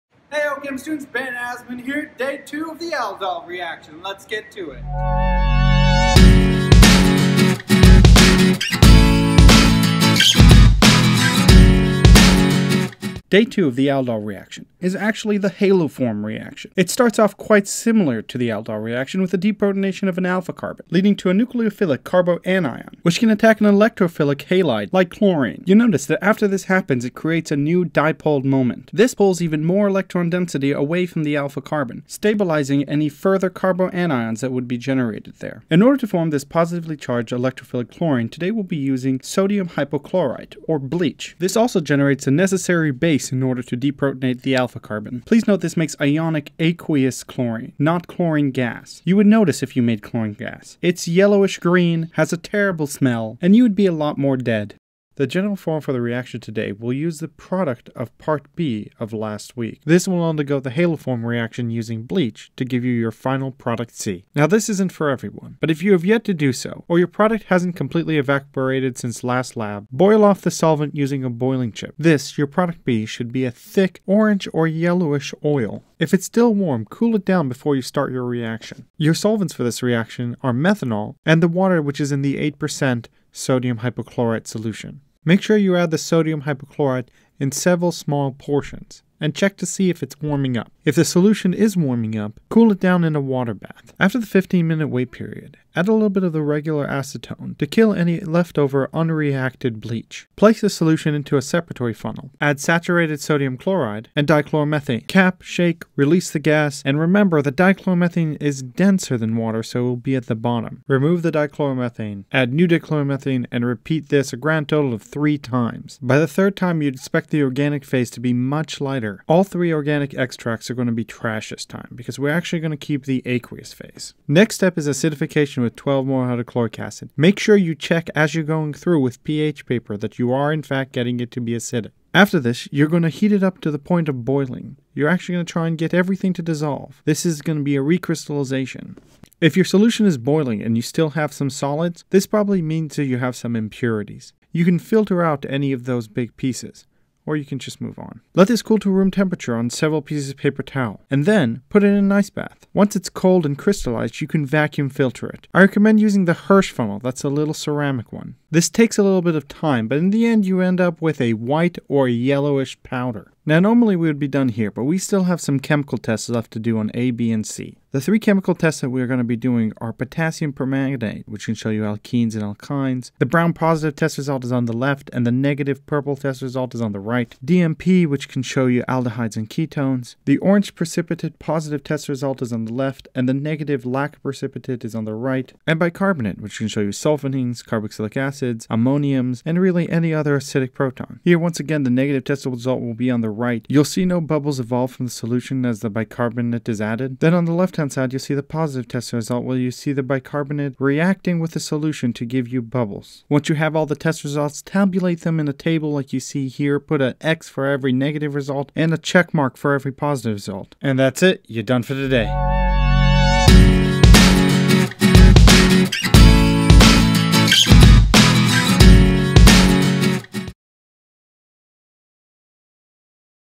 The Aldol Reaction part 2 (The Haloform Reaction)
Aldol Reaction Part 1

CSUN Chemistry and Biochemistry department
2nd Semester Organic Chemistry Lab
Mixed aldol condensation of benzaldehyde and acetone

Ref:
Chem 334 lab manual fall 2016
L.G. Wade, Organic Chem 8th

made by Benjamin D Azmon